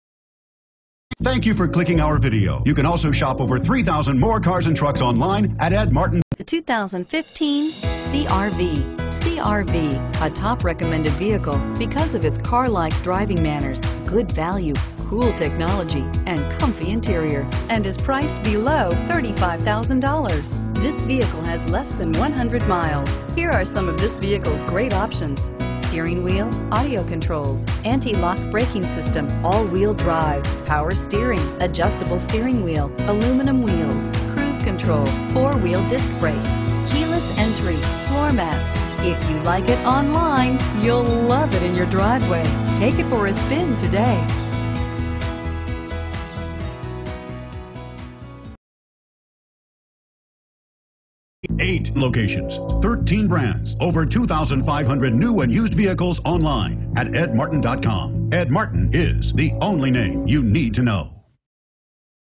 2015 HONDA CR-V Indianapolis, IN 2CR2875
2015 HONDA CR-V Indianapolis, IN
Stock# 2CR2875

The Honda CR-V has a modern aerodynamic look, with sweeping lines, exciting body sculpting, and a dramatic profile. It has enough attitude and ability to get you around town and then tackle the open road and the great outdoors. The CR-V comes equipped with a 2.4-Liter i-VTEC 4-Cylinder engine with 185hp and a Continuously Variable Transmission. It has a rigid unit-body structure, coupled with taut suspension tuning, which give it impressive handling characteristics. For ultimate 4-season security, the CR-V is available with Honda's Real Time All-Wheel Drive with Intelligent Control System that smoothes out initial acceleration and can transfer up to 100% of torque to the rear wheels. The CR-V is very fuel-efficient and can get up to an EPA estimated 34 MPG Highway when in two-wheel drive. Hit the green ECON button, and the vehicle makes adjustments to its systems so they can be more efficient. Then you get feedback as you drive from Eco Assist to let you know how efficient your driving is. Honda has placed bars around the speedometer, and the longer you keep them in the green, the farther you can go on a tank of gas. The 5-passenger CR-V is extremely roomy inside, with a rear seat that accommodates adults and a proper-sized cargo area. Special attention is paid to driver comfort, with supportive front seats that flank an accommodating center console punctuated by two big cup holders. A large covered center stow bin houses USB and auxiliary inputs as well as a 12-volt power point. Available lane keeping assist and adaptive cruise control add to assisted features. Finally, its large single-piece liftgate, coupled with a significantly lower lift-over height, eases loading and unloading chores.

All Wheel Drive, Power Steering, ABS, 4-Wheel Disc Brakes, Brake Assist, Aluminum Wheels, Tires - Front All-Season.